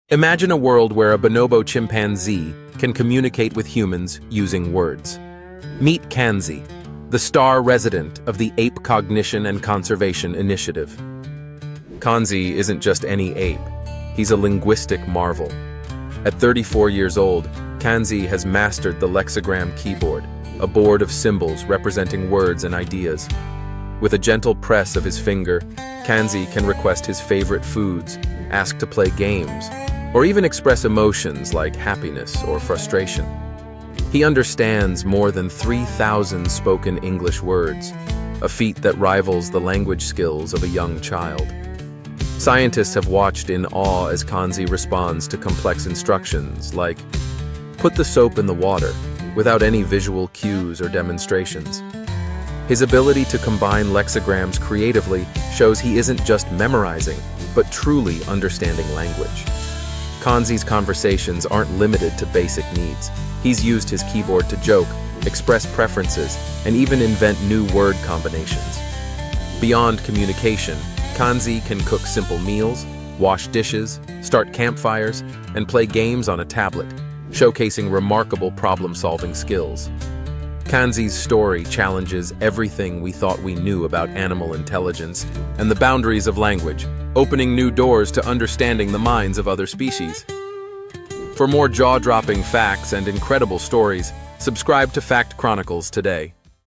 Kanzi The Chimp Who Can Cook And Do House Chores
Meet Kanzi, the bonobo who shatters boundaries! Discover how he uses a lexigram keyboard to communicate with humans.